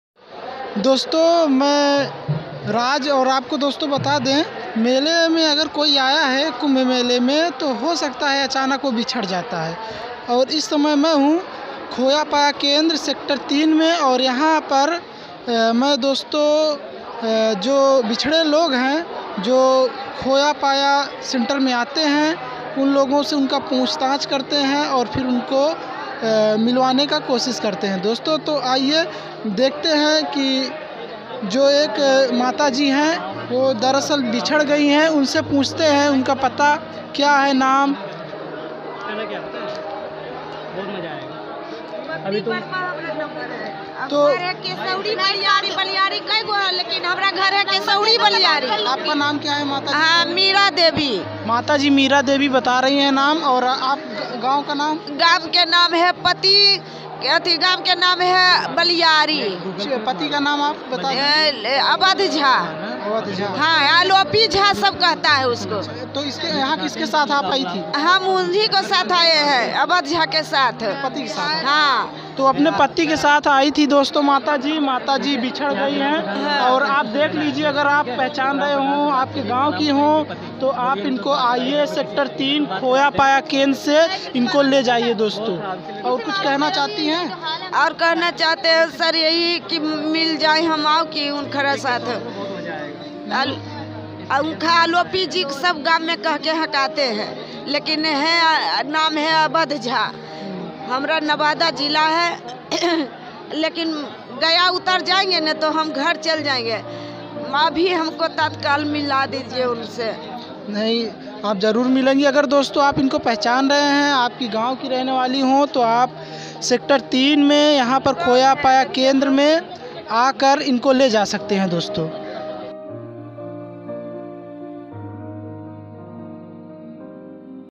Khoya Paya Center Of kumbh Mela Prayagraj 2019
इस वीडियो में कुंभ मेला में आए हुए जो भी भक्त खो जाते हैं उनके बारे में बताया गया है और उन लोगों को दिखाया भी गया है जो आकर इस मेला में अपनों से बिछड़ जाता है हमारा इस वीडियो को बनाने का मेन मकसद यह है कि यदि आप में से कोई व्यक्ति उस जगह से है उस गांव से है तो आप हमारी मदद कर सकते हैं इनको इनके परिवार से मिलाने मैं